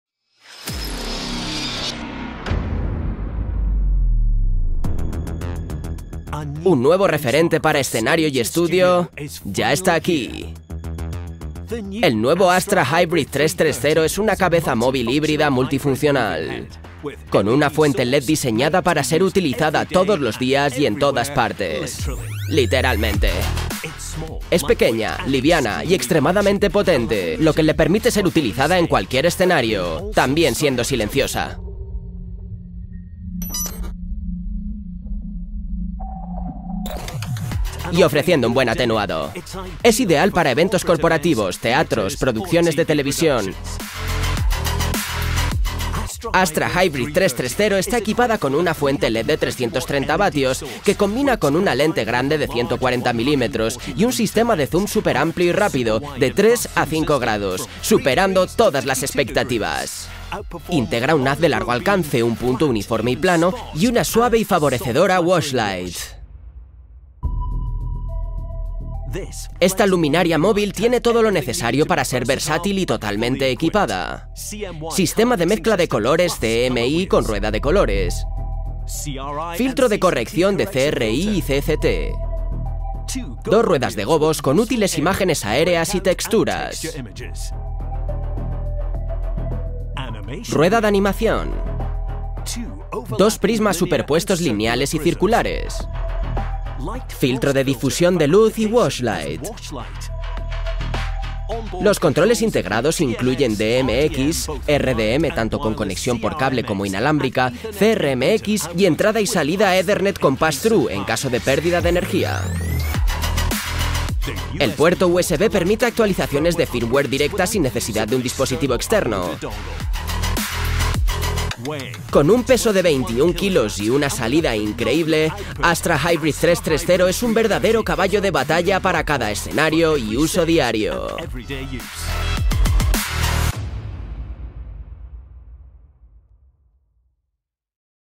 Cabeza móvil LED Astra Hybrid 330 de Proligts
Esta cabeza móvil LED híbrida destaca por su sorprendente potencia en comparación con su tamaño compacto, brindando una amplia gama de impresionantes efectos visuales.

El sistema óptico del Astra Hybrid330 es excepcional, con un rango de haz de luz de 3.5° a 52° y una lente frontal de 140 mm que proporciona una luz intensa y un enfoque uniforme. Esto se traduce en una salida de luminosidad de 8,158 lúmenes a 3.5° y 11,684 lúmenes a 52°, superando la uniformidad de las luces de descarga de arco en su categoría.

En el núcleo de esta unidad se encuentra un LED blanco de alta potencia de 330W, con una temperatura de color correlacionada (CCT) de 7,000K. A una distancia de 5 metros, puede generar una intensidad de 97,996 lux a 3.5° y 1,566 lux a 52°. Además, la fuente de luz tiene una vida útil de más de 30,000 horas.

El sistema de color del Astra Hybrid330 incorpora tres ruedas de mezcla de colores CMY que se desvanecen gradualmente. También incluye una rueda de temperatura de color con opciones de 2,400 K y 3,200 K. Además, cuenta con 9 filtros dicroicos distribuidos en tres ruedas de colores independientes, así como filtros de color con alto índice de rendimiento cromático (CRI).

En cuanto a los efectos dinámicos, el Astra Hybrid330 cuenta con un dimmer de 16 bits para ajustes precisos de intensidad de luz. Incorpora una rueda de animación con rotación en ambos sentidos. Además, ofrece 8 gobos rotativos y 11 gobos fijos intercambiables e indexables. También incluye un generador de efectos FX con dos prismas superpuestos: un prisma circular de 8 facetas y un prisma lineal de 6 facetas, ambos con rotación bidireccional e indexación. Asimismo, se incluye un filtro frost lineal del 0 al 100% para suavizar y difuminar la luz.

El cuerpo del Astra Hybrid330 está fabricado en aluminio y cuenta con una cubierta de policarbonato altamente resistente. Puede moverse en el eje Pan en un rango de 540° y en el eje Tilt en un rango de 270°, con una resolución de 8/16 bits. En caso de un movimiento accidental, cuenta con un sistema de reposicionamiento automático.